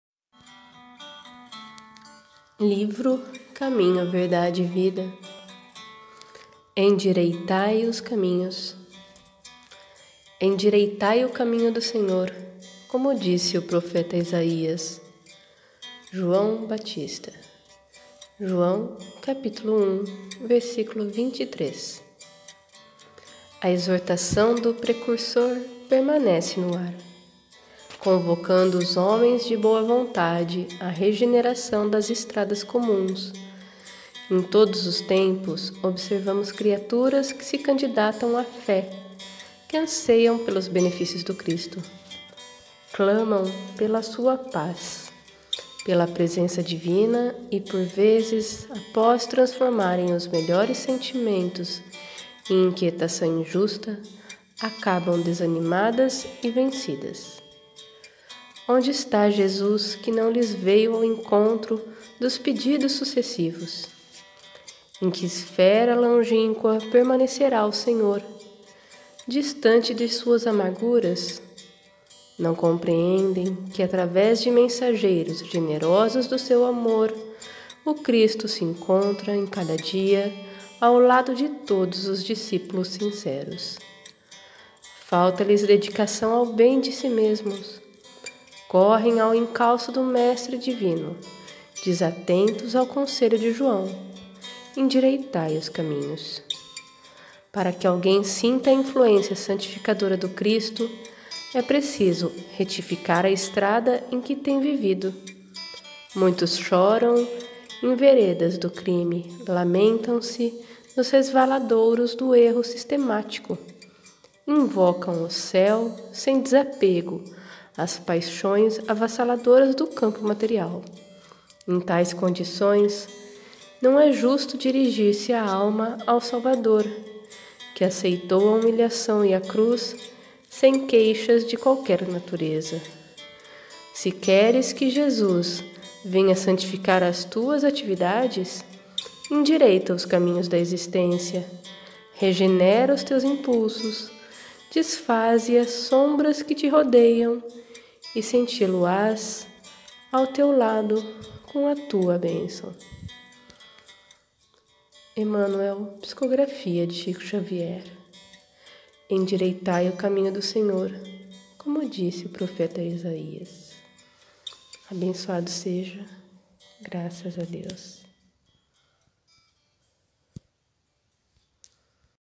ENDIREITAI OS CAMINHOS - Livro: Caminho, verdade e vida. Emmanuel, por Chico Xavier
Siga nosso canal do YouTube e o Instagram @fazer.o.bem.sempre 🙌❤❤❤

leitura 02/07/2021
livro: Caminho, verdade e vida
Autora: Emmanuel
Psicografia de Chico Xavier

☝️Lembra: é possível fazer o bem sempre como e onde estiver!